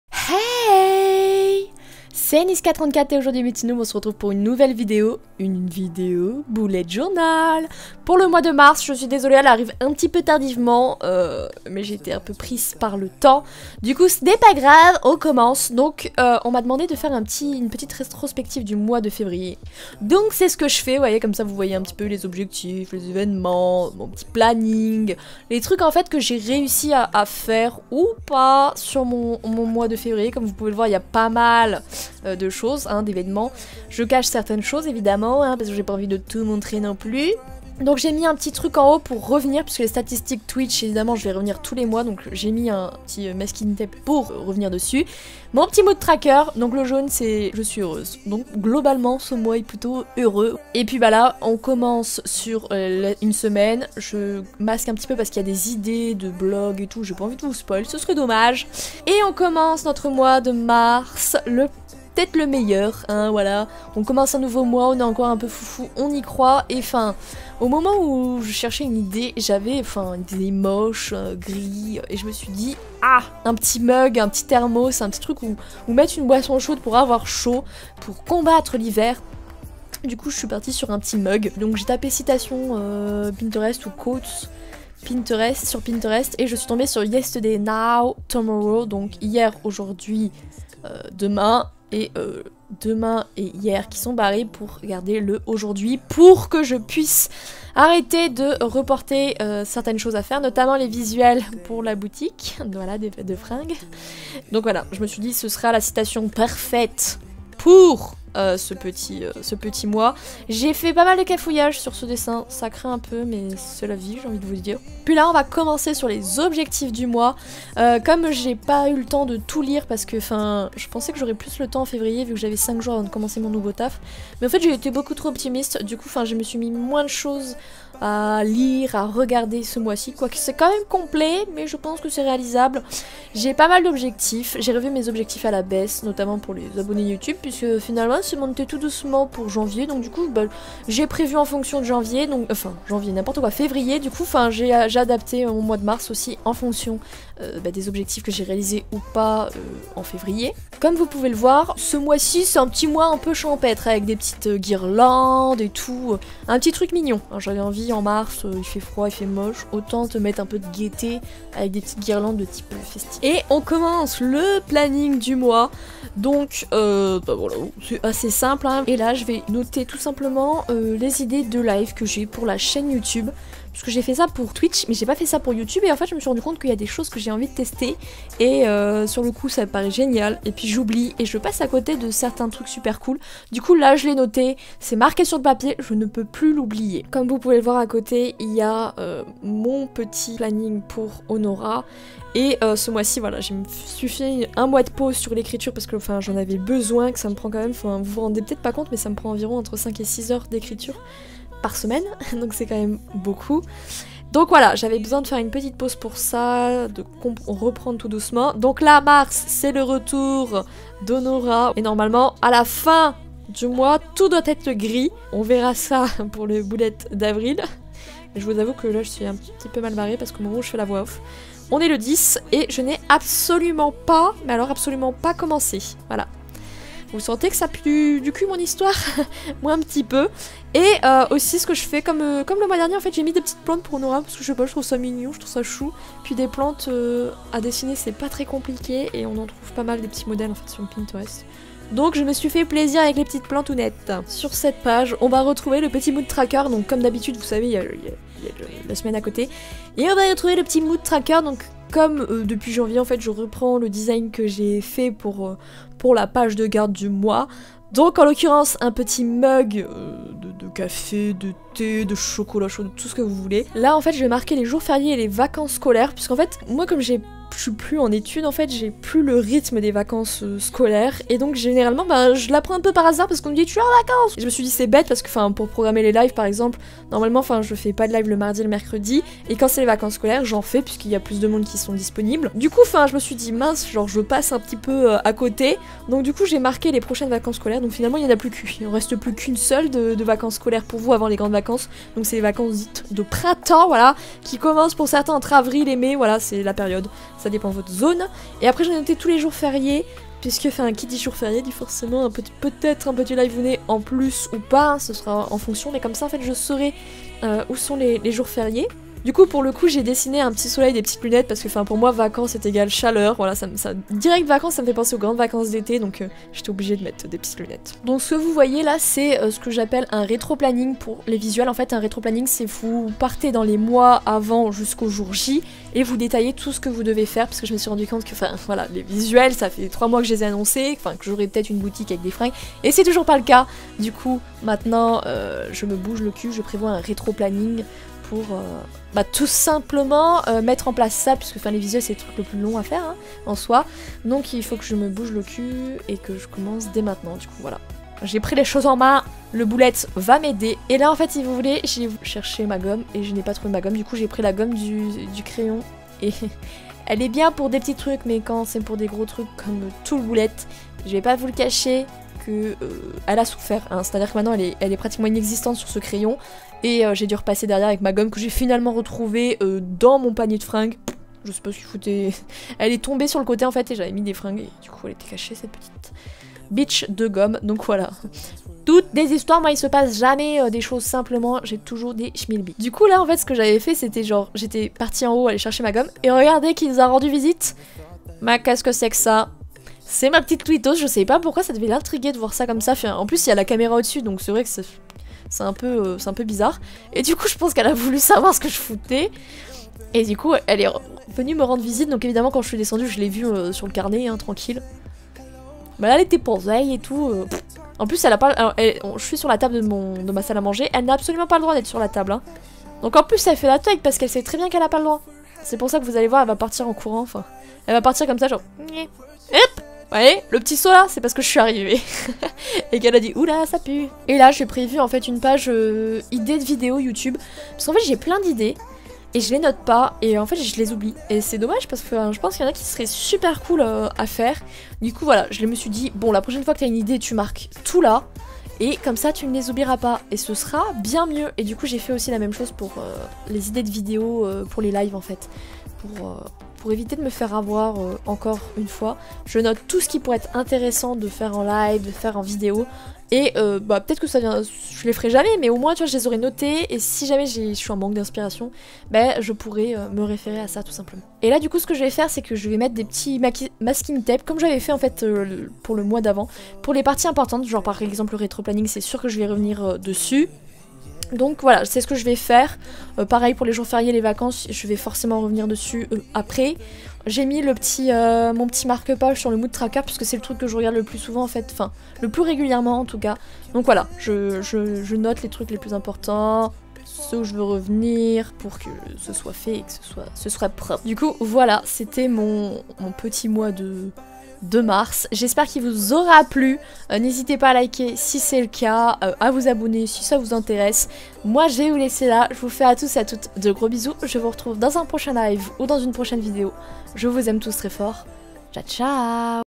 Bullet Journal Mars 2018 : Plan with me
Hey c'est Nyska34 et aujourd'hui mes petits noobs on se retrouve pour la vidéo bullet journal de mars 2018 !
Bon ça va, on est pas encore le 15 du mois, donc je ne suis pas en retard !
J'espère que ce Plan with me spécial Bullet Journal vous plaira !

Wattpad : Nyska34
Galerie Sims 4 : Nyska34

Nombre de noobys : 17 423